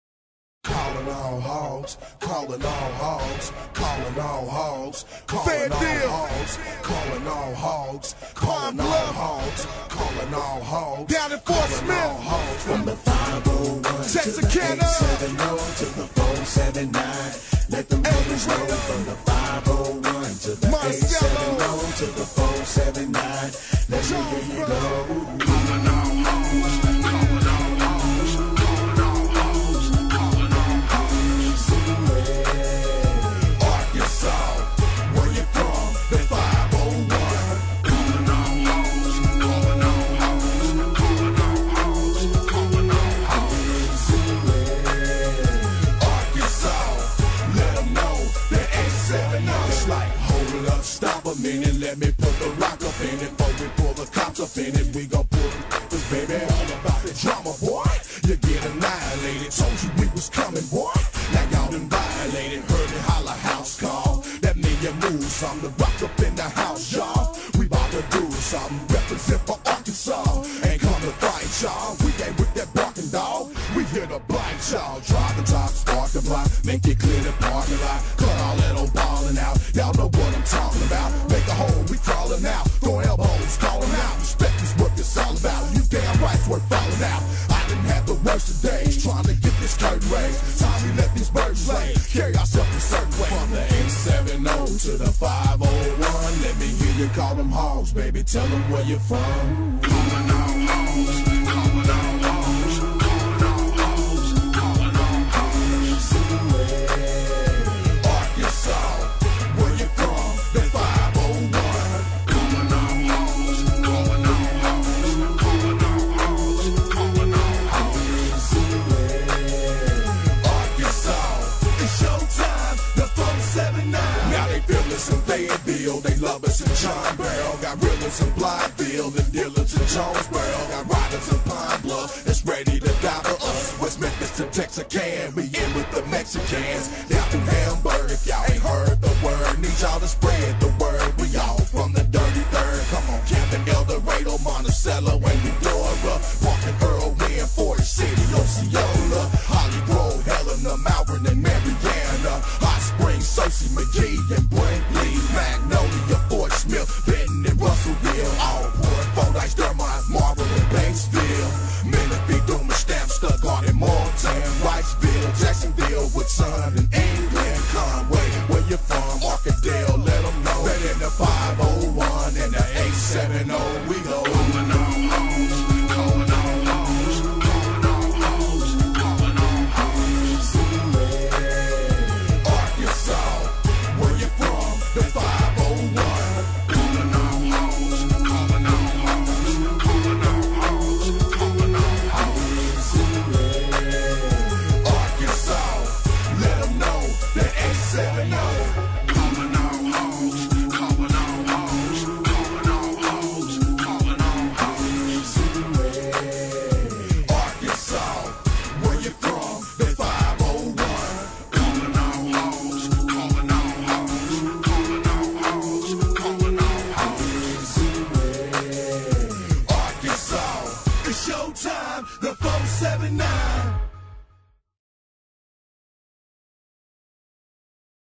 Razorback Baseball 06: Callin' All Hogs
Video made for EA Sports package to include Baum Stadium and the Razorbacks.